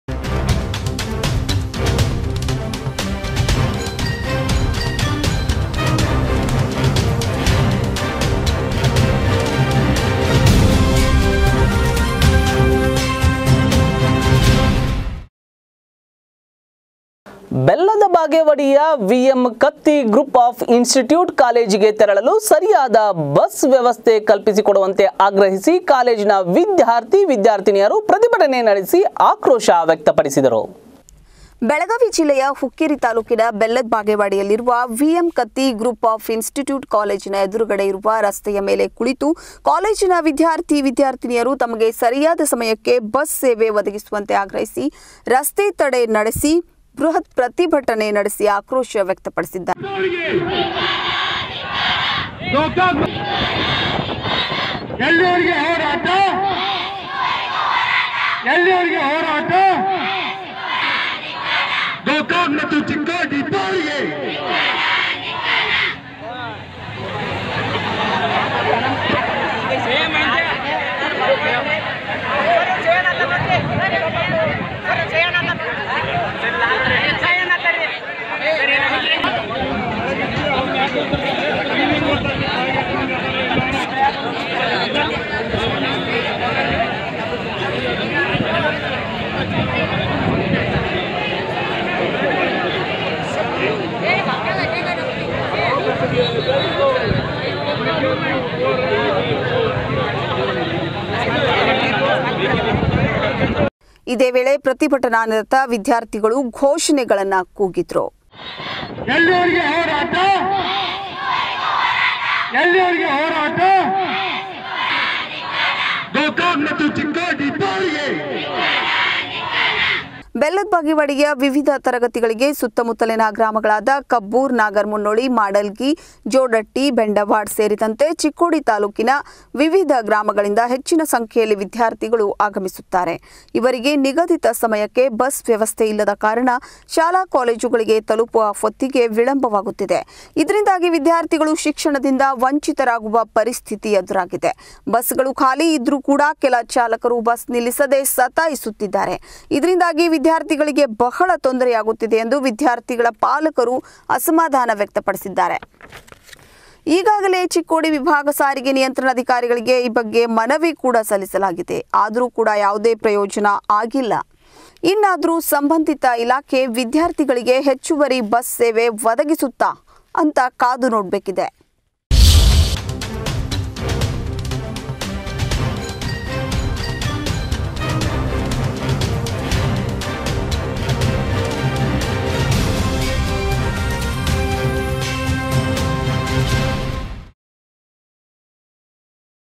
ಬಸ್ ಕಲ್ಪಿಸಿಕೊಡುವಂತೆ ಆಗ್ರಹಿಸಿ ಕಾಲೇಜಿನ ವಿದ್ಯಾರ್ಥಿ ವಿದ್ಯಾರ್ಥಿನಿಯರು ಪ್ರತಿಭಟನೆ ||SATYAM TV KANNADA
ಬಸ್ ವ್ಯವಸ್ಥೆ ಕಲ್ಪಿಸಿಕೊಡುವಂತೆ ಆಗ್ರಹಿಸಿ ಕಾಲೇಜಿನ ವಿದ್ಯಾರ್ಥಿ ವಿದ್ಯಾರ್ಥಿನಿಯರು ಪ್ರತಿಭಟನೆ ||SATYAM TV KANNADA ||